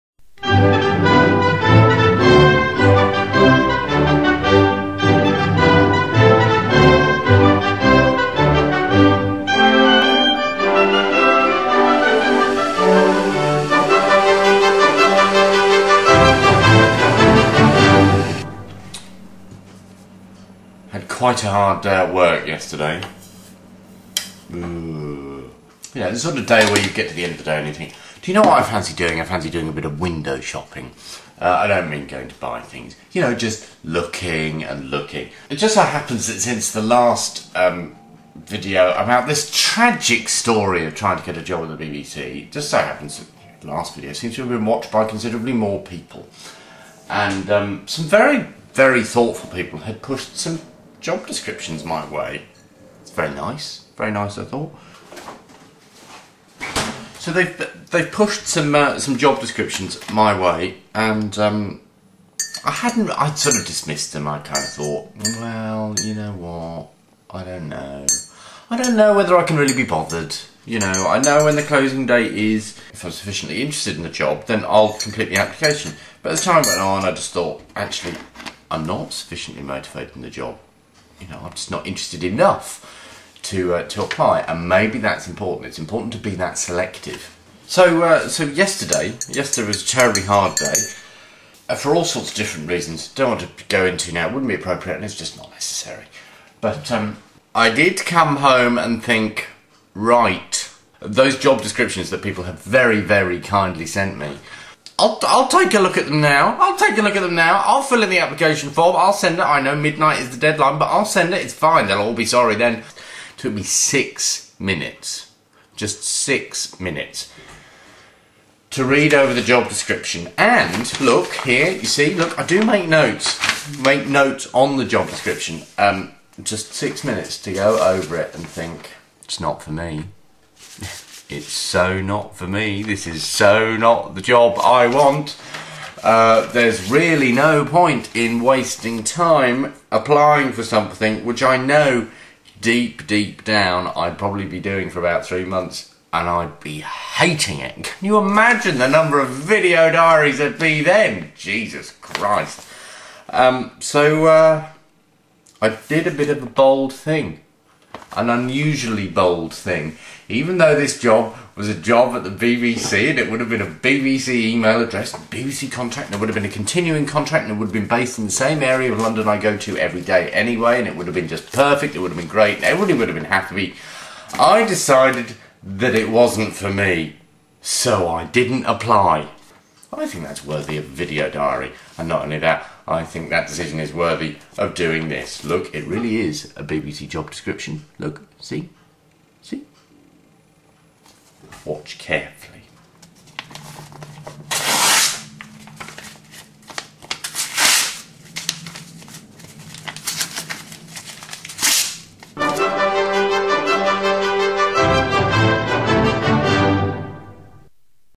Thoroughly Good : Yet More Advice
The latest in this tragic tale about getting a job at the BBC